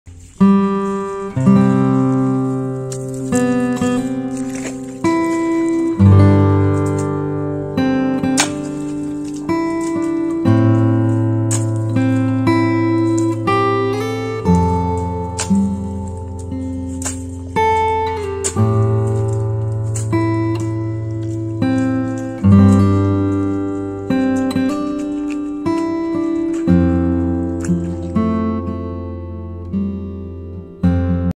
Traditional Farmer : in my village this activity is called 'mopok galeng'
Rice seeds sown in the field, about 20 days old, preparation for planting rice, the first step is spraying pests to prevent pests from arising,